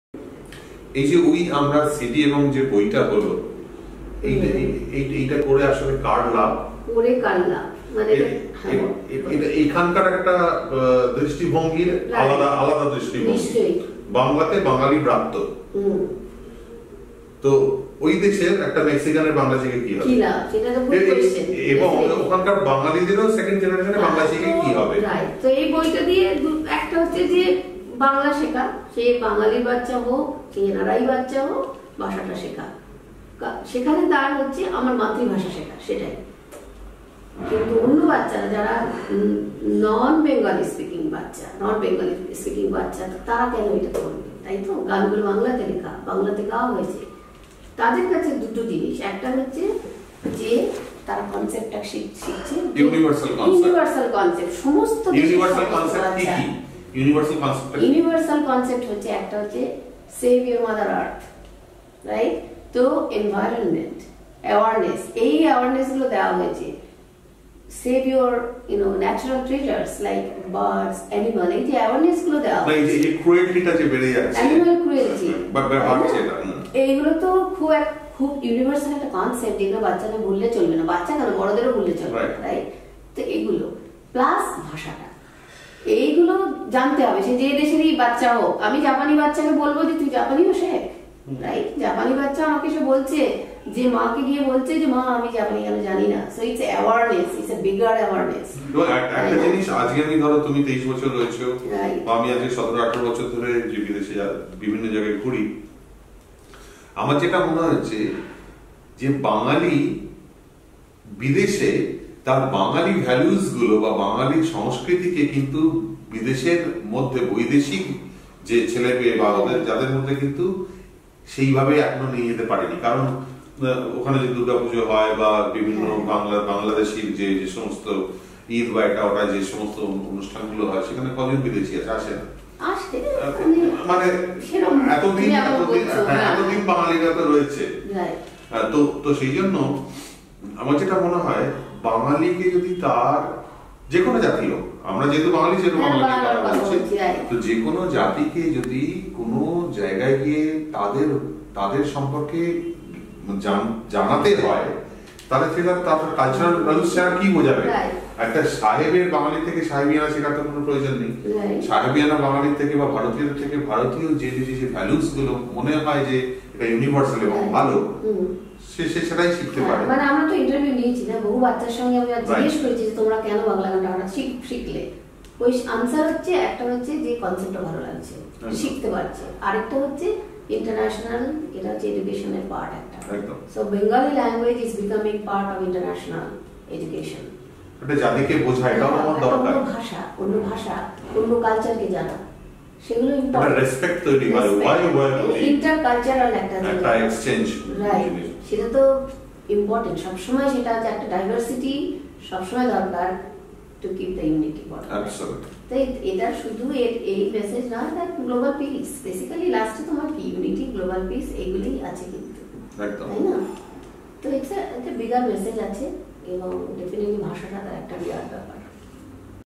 WE AMRA JOURNEY VIDEO 10; SOME DISCUSSION BETWEEN AUTHOR AND MUSIC COMPOSER OF WE AMRA PART 1
We-Amra Part 1: to provide education to all children in the form of entertainment. It has two major universal learning goals. “Love your mother earth” and “Love your mother tongue.” This provides a great opportunity to all NRI Bengali children to learn their mother tongue and culture through music and arts. This is also provides a good opportunity to other (not Bengali speaking children) children to learn these universal concept in a joyful manner though a new language.  We-Amra project helps to get connected with people all over the world and provides opportunity to learn from each other and exchange thoughts and views and ultimately makes a bride of friendship and love.